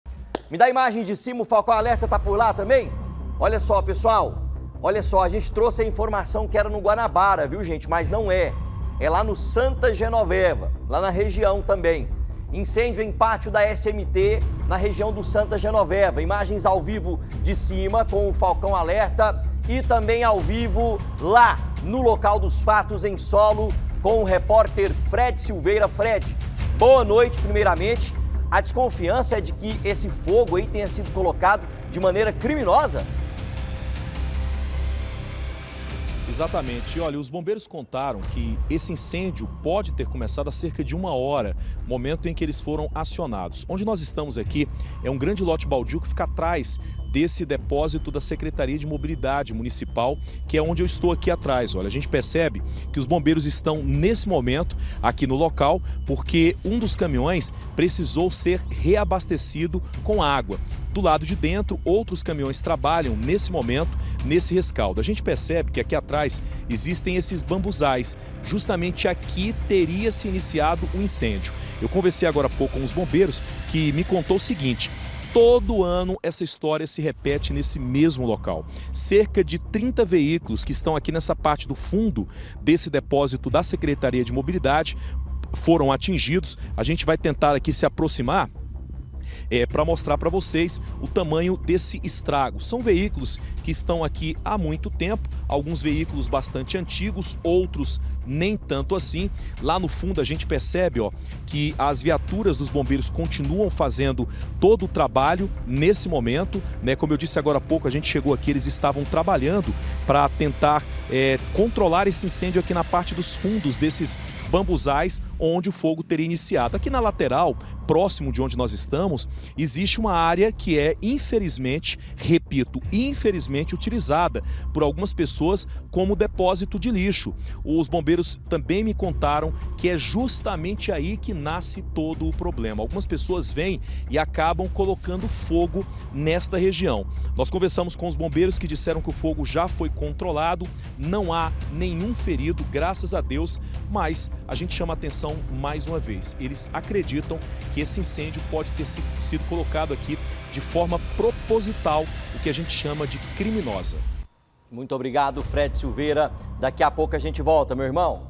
FOGO NO PÁTIO DA SMM PODE TER SIDO CRIMINOSO
INCÊNDIO EM PÁTIO DA SMT, NO SANTA GENOVEVA A INFORMAÇÃO É QUE O FOGO COMEÇOU NO BAMBUZAL AO LADO E PASSOU PRO PÁTIO.